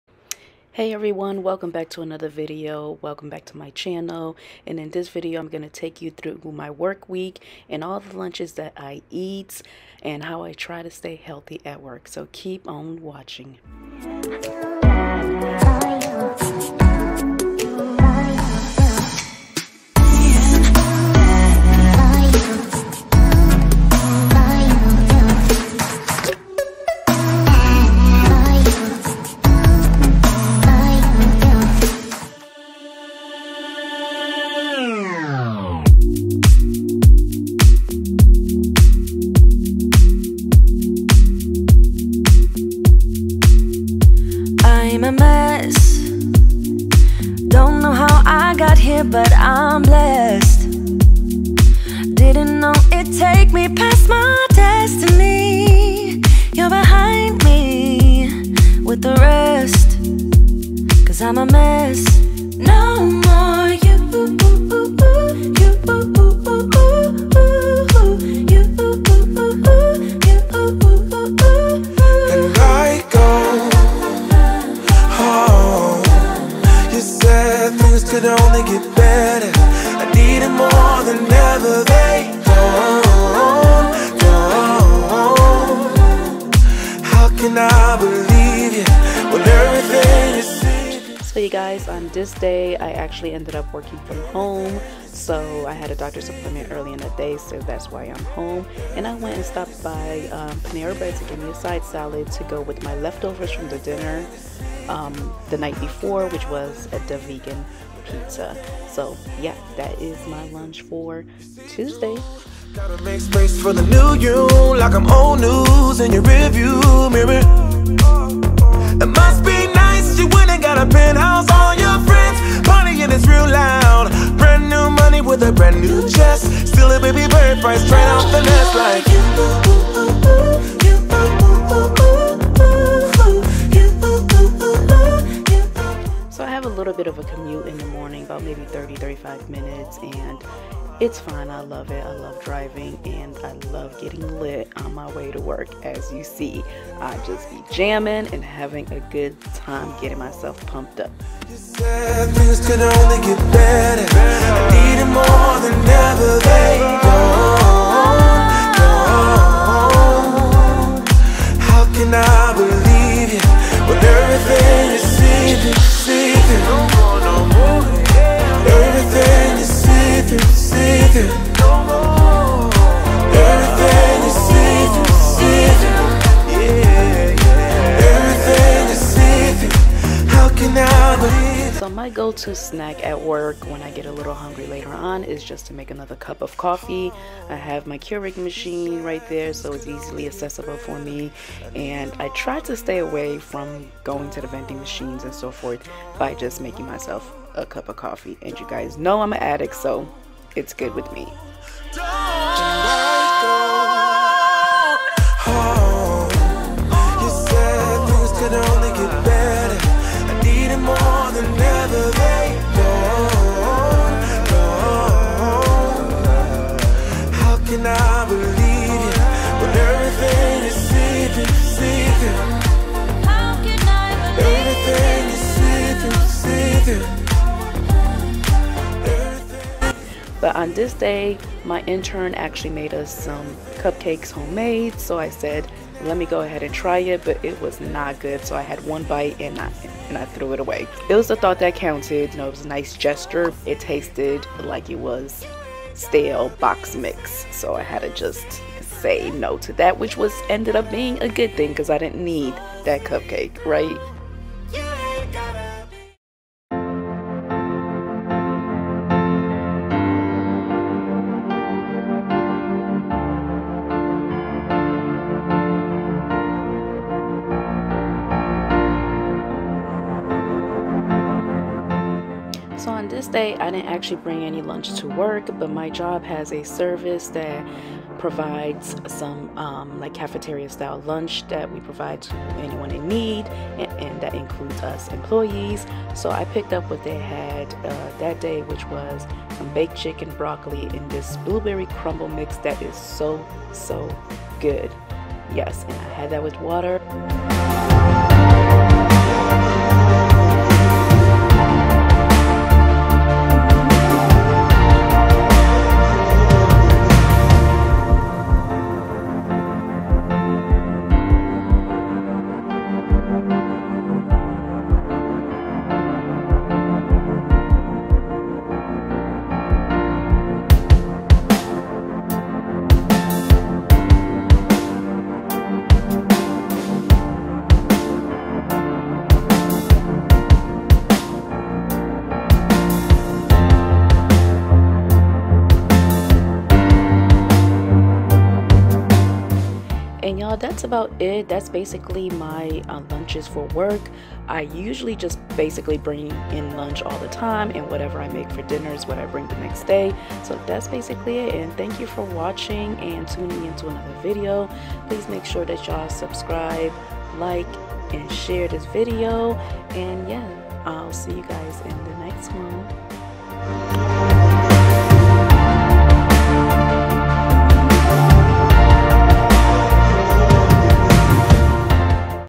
5 HEALTHY LUNCH IDEAS | 9 - 5 WORK LIFE!
Hey Healthy Living Fam,

In this video, I'll be taking you with me to work all week long and showing you what 5 healthy lunch meals and how I maintain my healthy lifestyle at work.